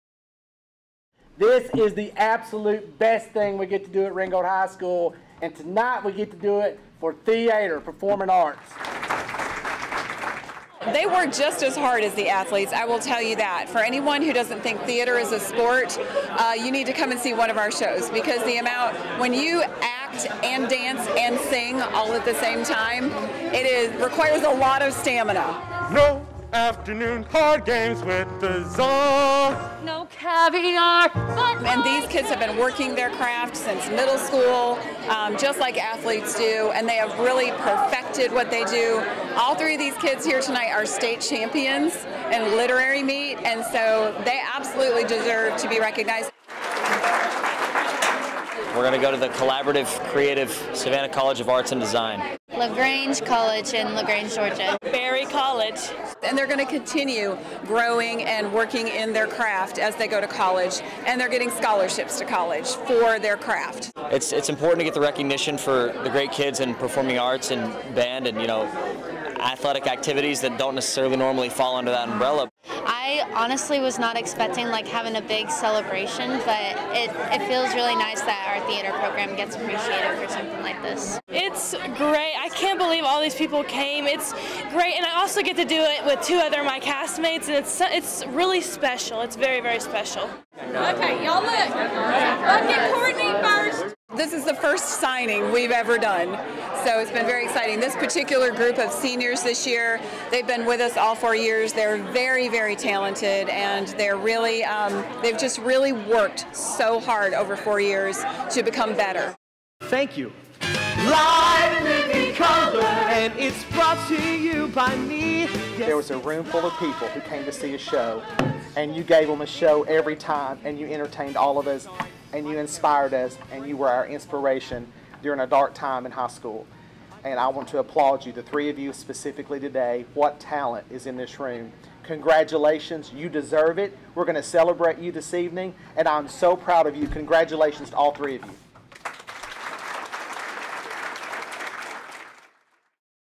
WRWOS: Ringgold Theatre
What if theater kids got a signing day like athletes? They do in Ringgold